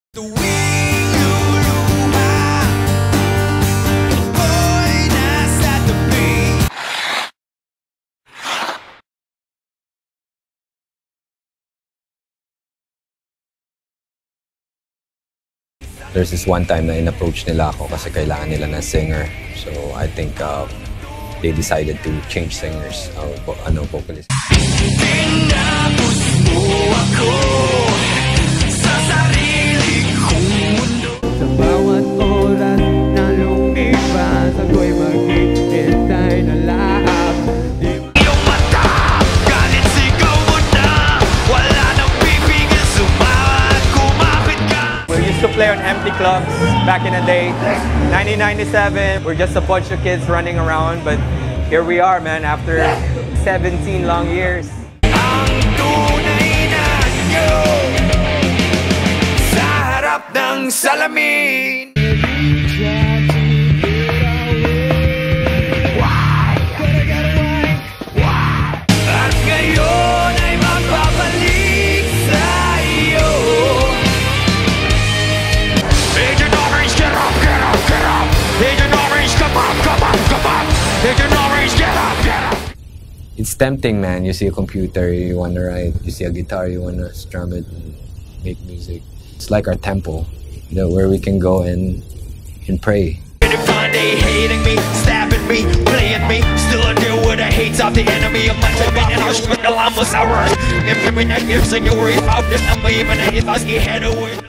Tribute to Jamir Garcia | Slapshock
RIP Vladimir "Jamir" A. Garcia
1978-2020
On November 26, 2020, Jamir Garcia was found dead at his home as the cause of an alleged suicide. His live-in partner Sojina-Jaya Crisostomo found Garcia's body hanging on a steel window using blanket around his neck inside his home's comfort room.